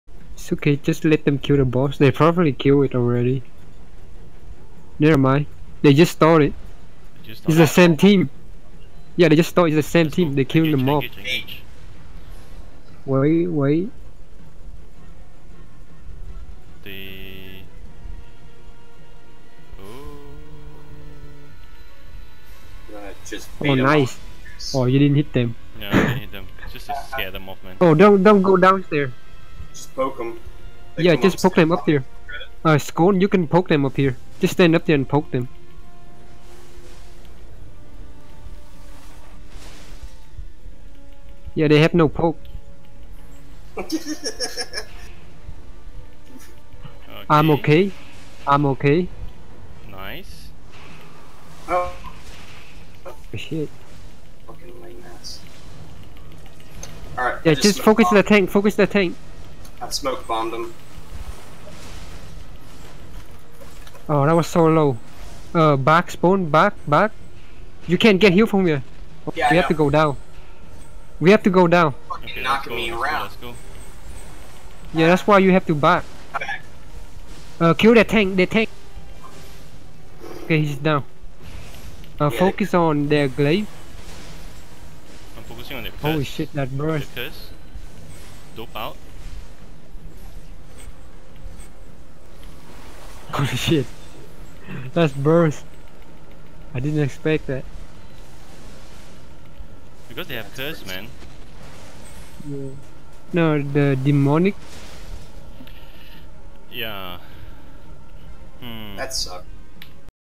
Inside Yellow HG #041
Unedited - only cropped to get to the fights.

In accordance to the party (as per my view)
VanguardReagan - Frost
KayNagebinary - Spear
Sporn - Quarterstaff
QARO - Holy
SkOn - Crossbow